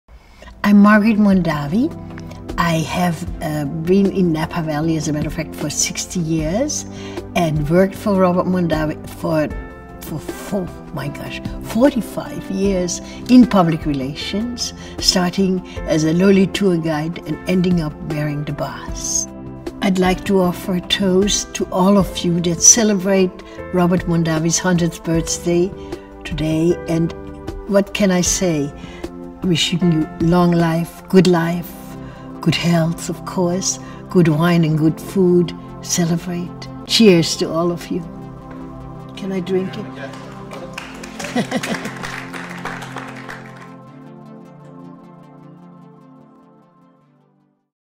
Celebrate the 100th anniversary of Robert Mondavi's birth
Cheers to Mr. Mondavi! June 18 is the 100th anniversary of the birth of winemaker Robert Mondavi, and we're celebrating with great deals on his wines.